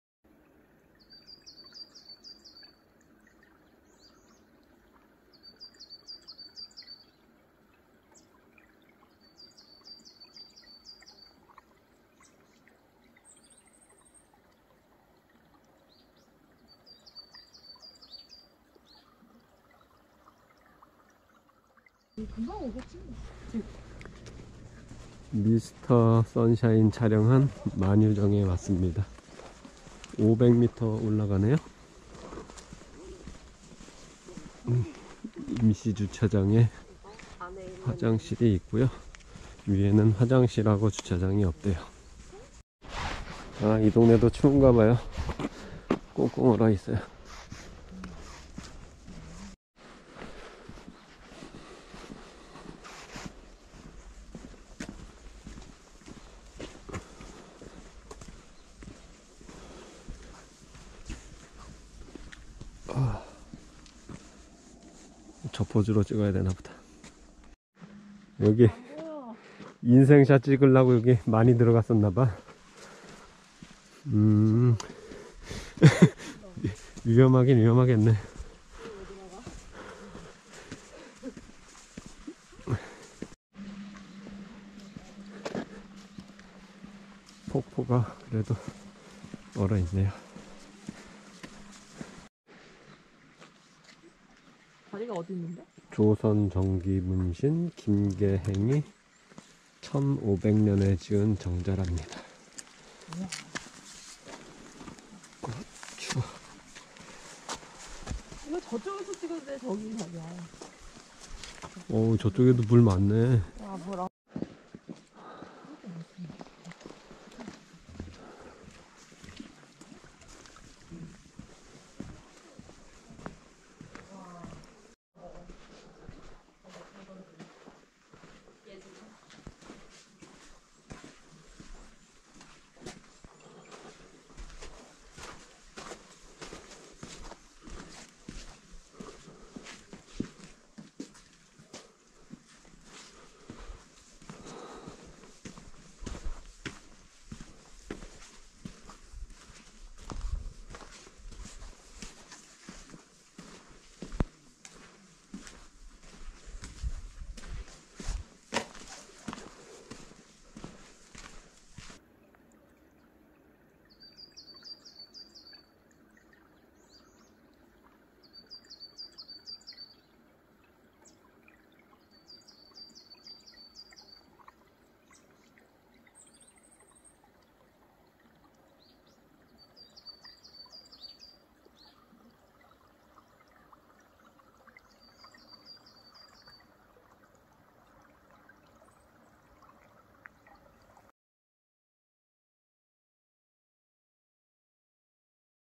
안동여행 - 미스터션샤인 촬영지 만휴정 겨울풍경
미스터션샤인 촬영지인 만휴정에 들려 봤습니다.
임시주차장에 주차하고 500m 정도 올라가면 있는 정자입니다.